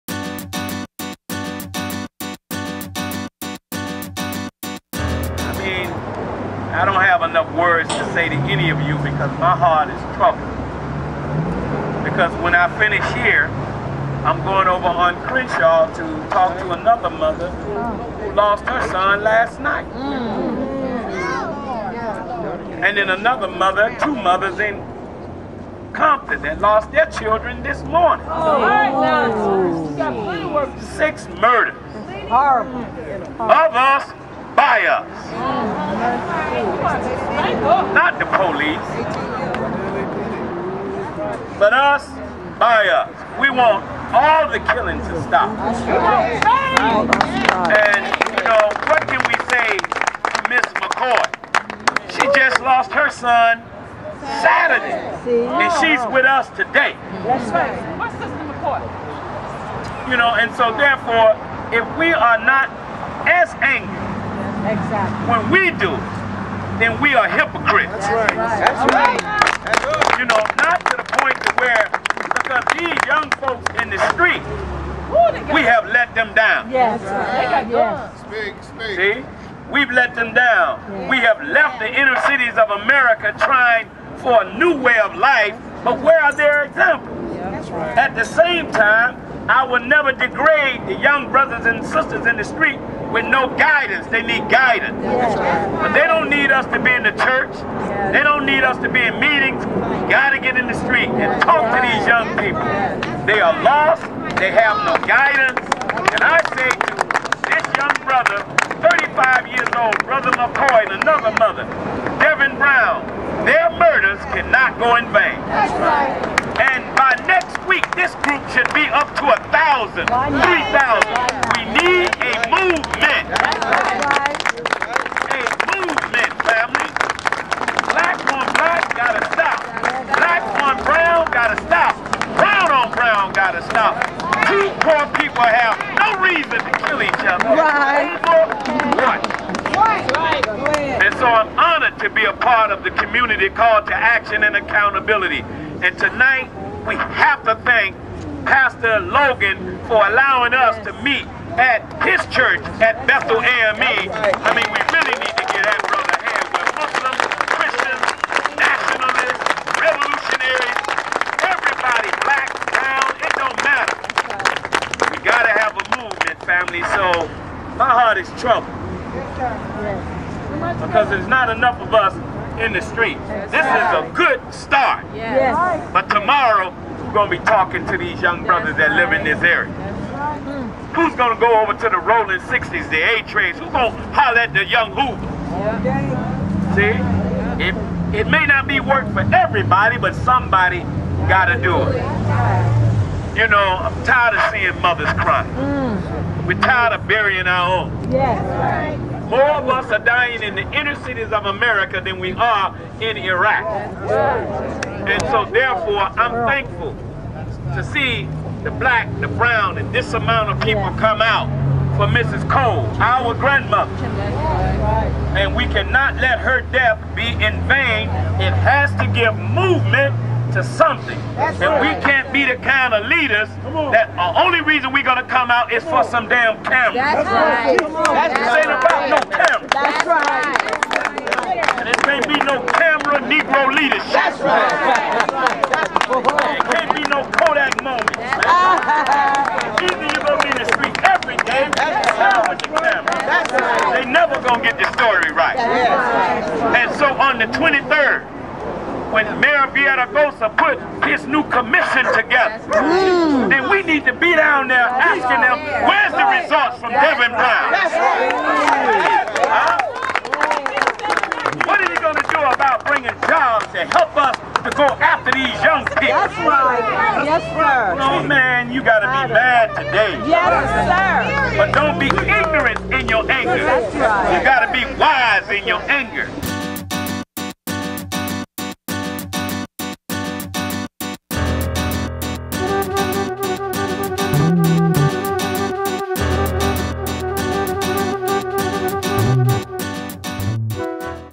Minister Tony Muhammad stop the violence
Minister Tony Muhammad hits the street to help solve the problems in Los Angeles